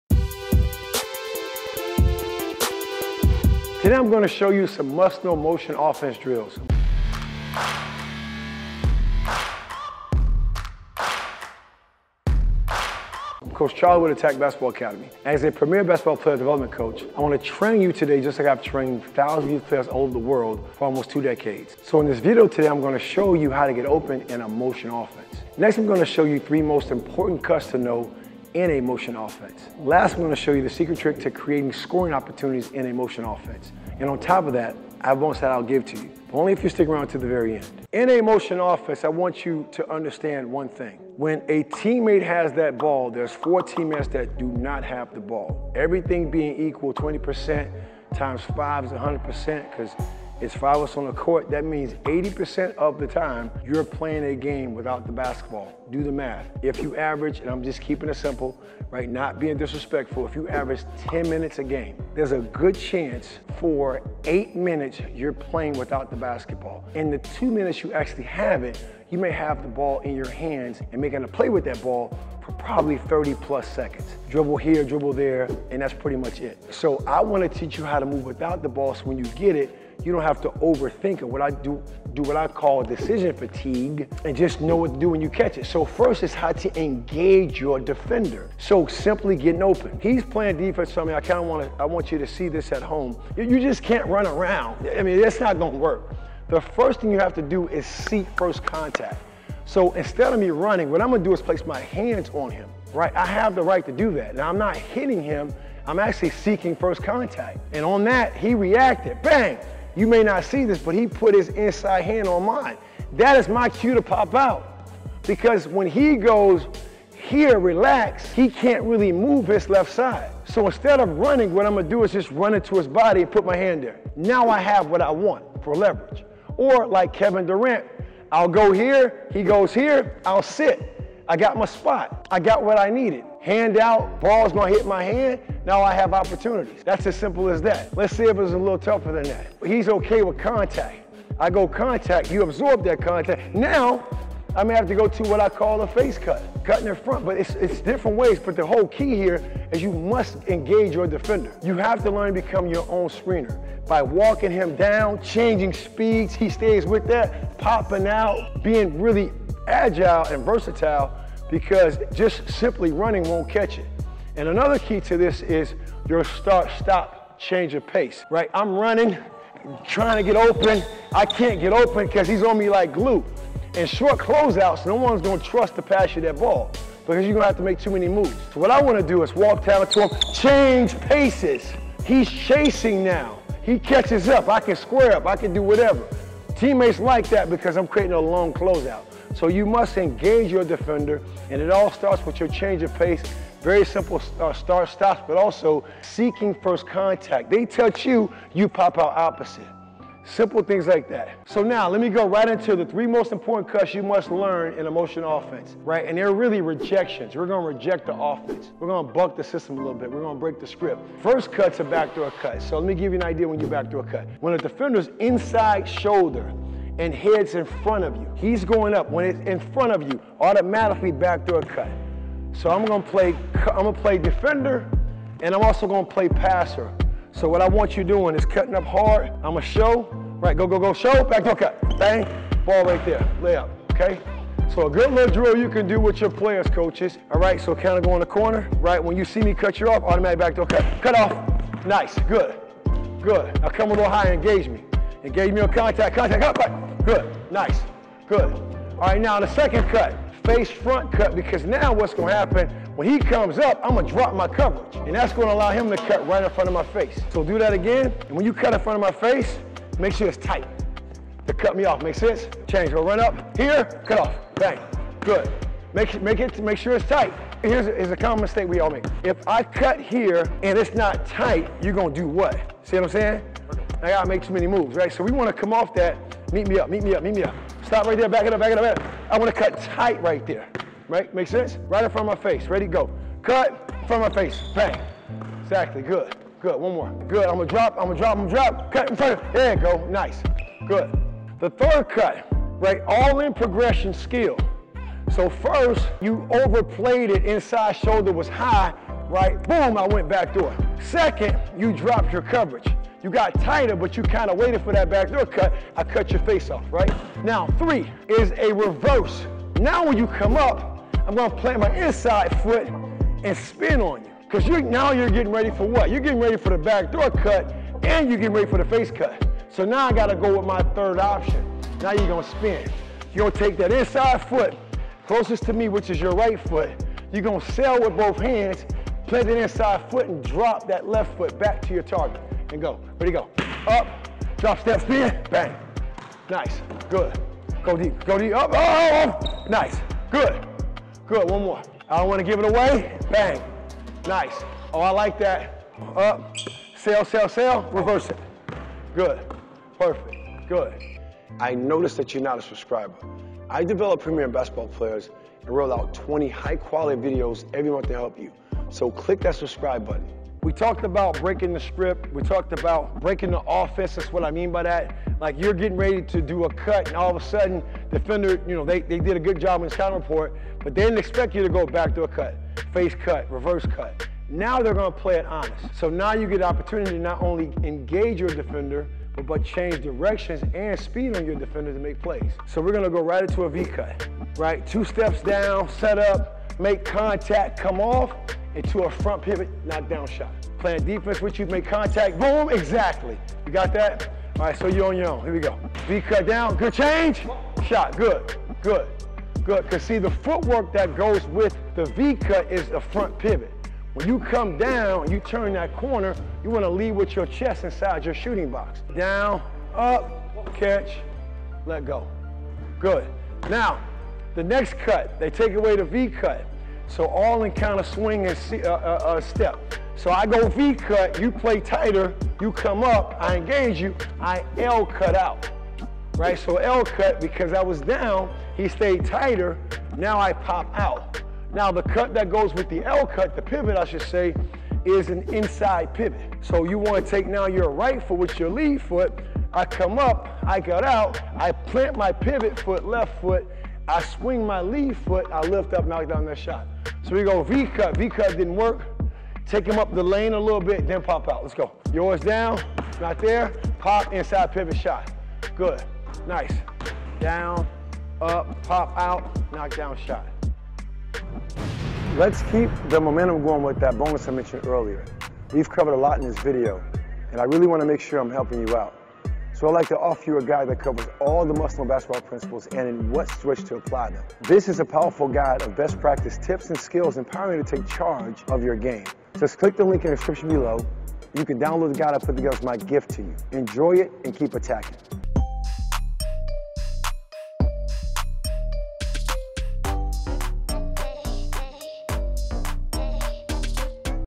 Motion Offense Drills
80% of the time in any basketball game, you're not going to have the basketball in your hands on offense, so it's important to know what to do when you do not have the ball to be a good asset to your team. Master Player Development Coach, Charlie Miller is internationally known as a 5-star coach and has helped thousands of players around the world by using these same techniques. Don't miss this video!

00:00 Intro & Summary
00:37 How To Get Open In Motion Offense
03:21 3 Most Important Cuts To Know In A Motion Offense
06:00 The Secret Trick To Creating Scoring Opportunities In A Motion Offense
07:42 BONUS GIFT